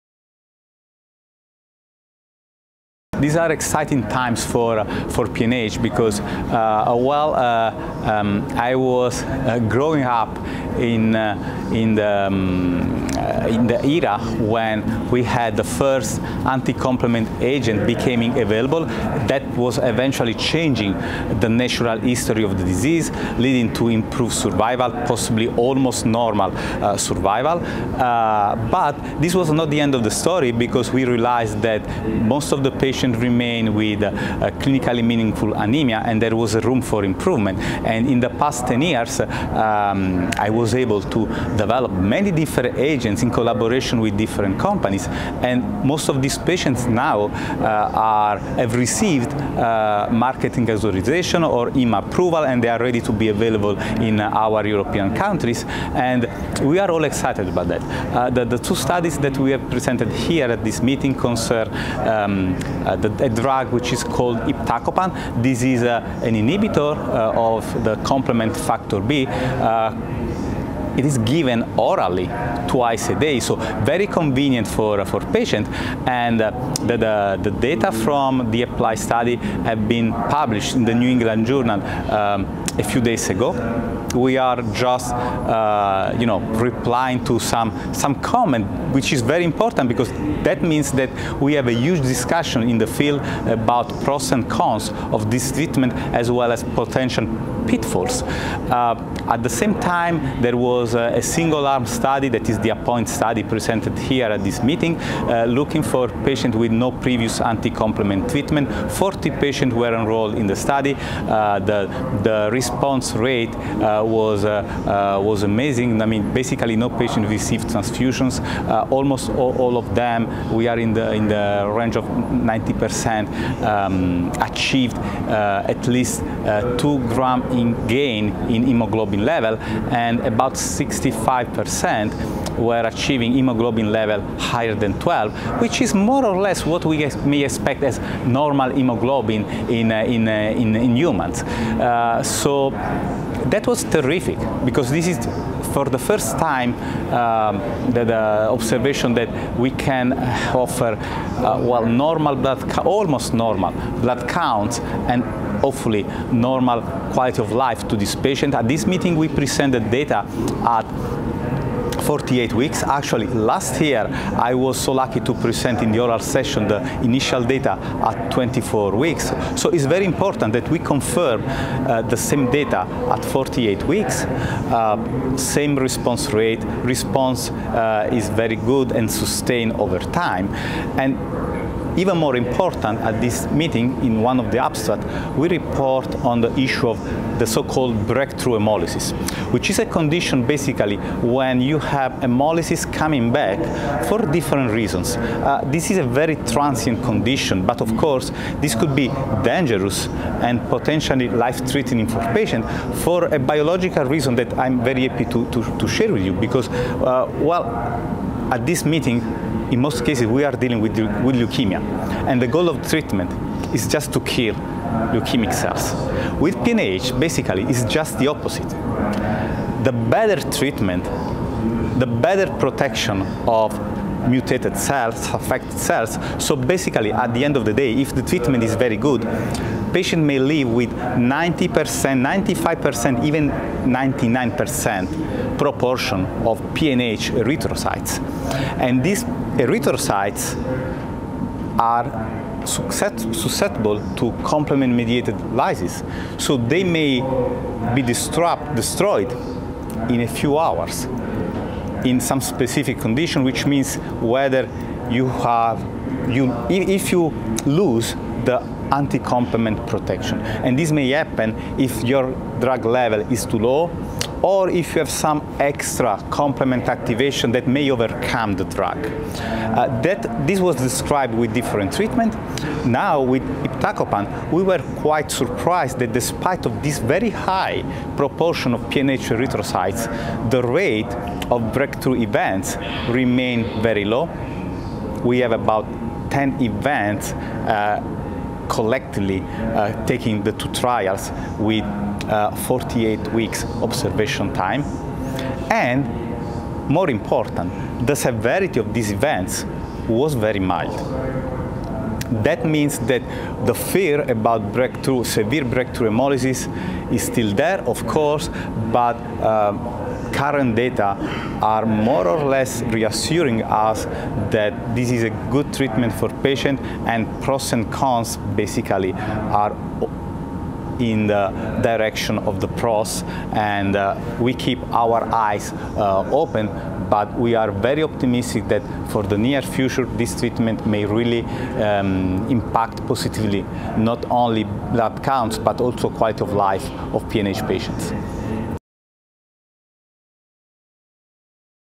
Complement inhibition with iptacopan has high efficacy in PNH: insights from APPLY and APPOINT
Antonio Risitano, MD, PhD, AORN San Giuseppe Moscati Di Avellino, Avellino, Italy, presents encouraging data on the anti-complement agent iptacopan for treating paroxysmal nocturnal hemoglobinuria (PNH), as evidenced by the APPLY-PNH (NCT04558918) and APPOINT-PNH (NCT04820530) trials. In the APPOINT trial, sustained responses, with hemoglobin levels nearing normal in some patients, were observed after 48 weeks. Dr Risitano also addresses the issue of breakthrough hemolysis (BTH) associated with complement inhibitors which has been rare and low severity in these trials. This interview took place at the 50th Annual Meeting of the EBMT in Glasgow, Scotland.